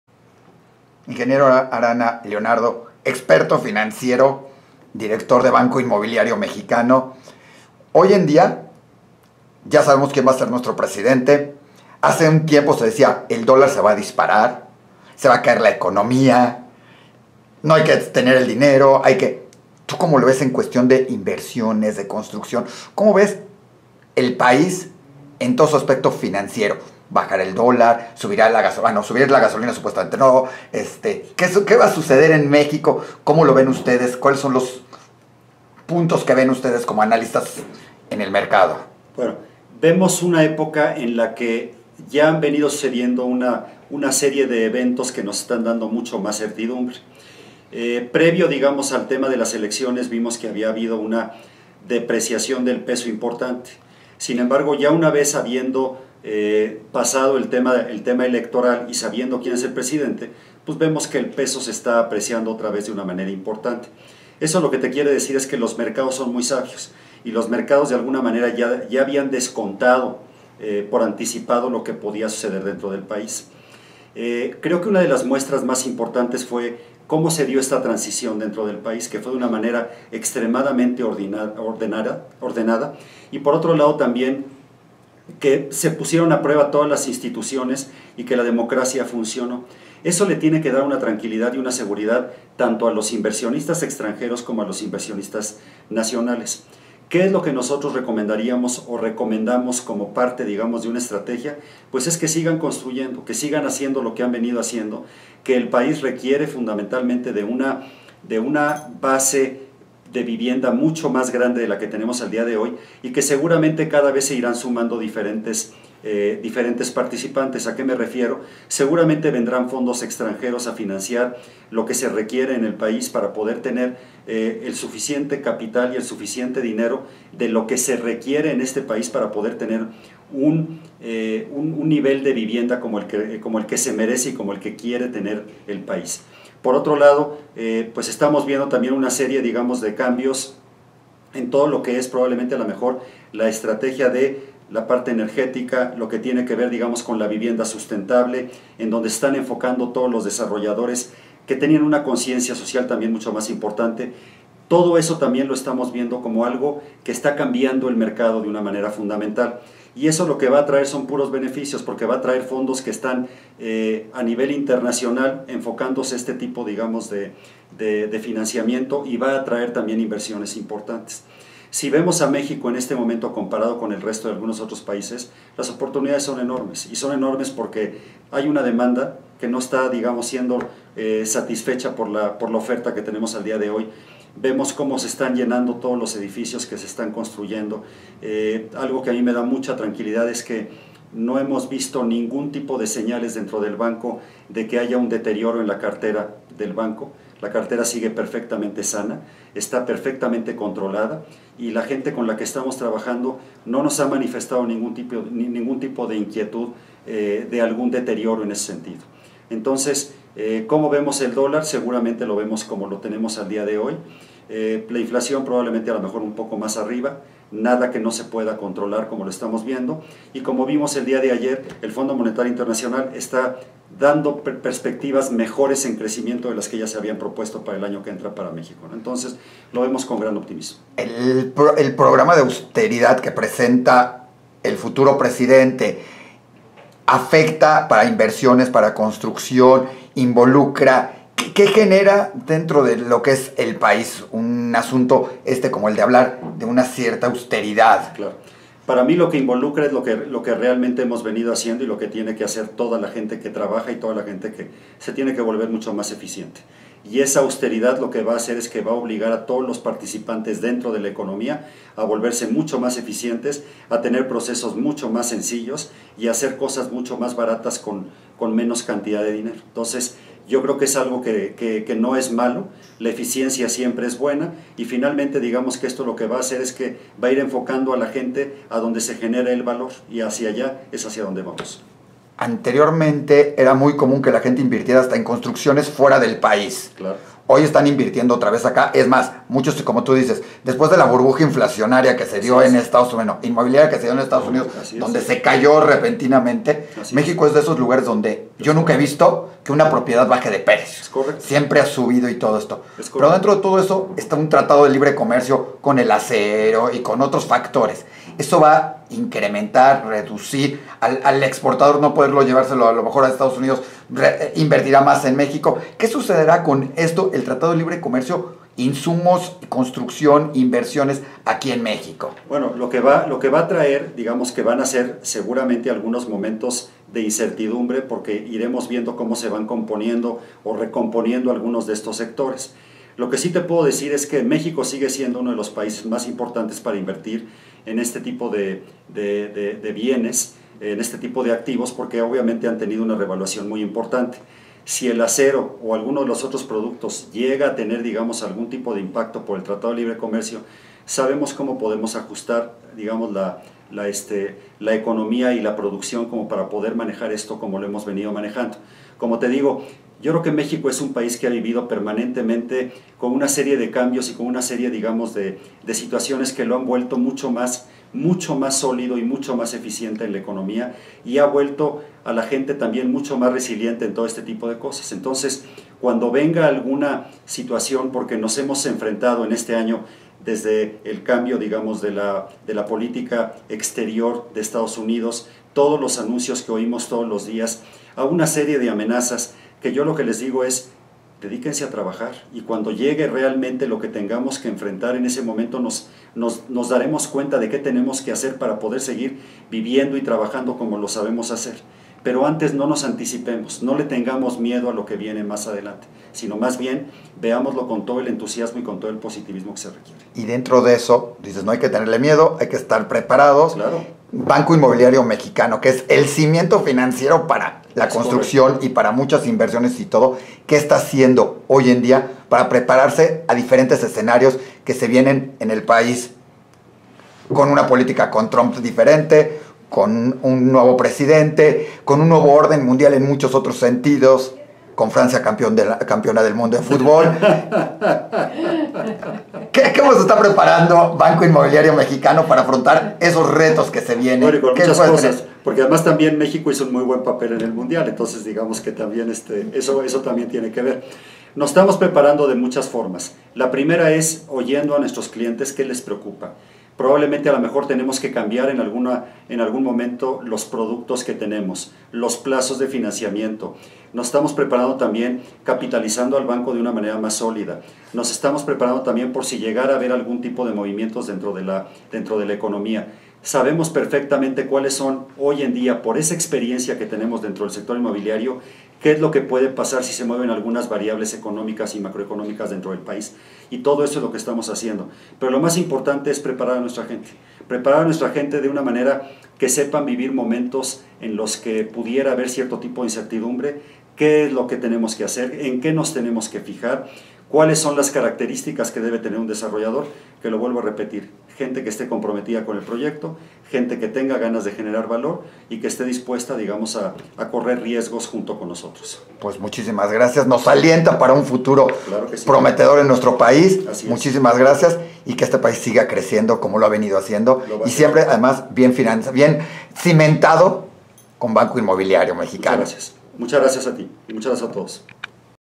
Expectativas finacieras y economicas de México para este 2018 y 2019 BIM 6
Como invertir en construcción correctamente  y las Expectativas financieras y económicas para este 2018 y 2019  BIM 3
Experto en crédito e inversiones, Leonardo Arana, Director General de Banco Inmobiliario Mexicano nos explica como invertir, como conseguir y que crédito Usar
Como invertir en construcción correctamente  y las Expectativas financieras y económicas para este 2018 y 2019
El Ingeniero Arana Director del Banco inmobiliario Méxicano nos explica el como porque y como ve a México en estos momentos y las expectativas que se esperan con un nuevo Gobierno
Ademas nos explica la importancia de realizar un estudio de factibilidad, el saber si se tiene o no experiencia y como apoyarle en todo momento, ya sea que tiene el interes,, tiene un proyecto un terreno, lo importante no es tener los recursos sino el valor para una alianza estrategica con BIM
Ademas de los beneficios que tiene el trabajar con una institución tan solida y con todos los servicios fiduciarios, y asesoría como lo es BIM  de grupo Hir
YU cual sería el consejo mas importante para los desarrolladores , constructores, y todos los que están en la parte de Bienes raíces construcción o similares en el país

Diario de la vida judía en México y el MundoPara mas videos y materiales muy interesantes
visita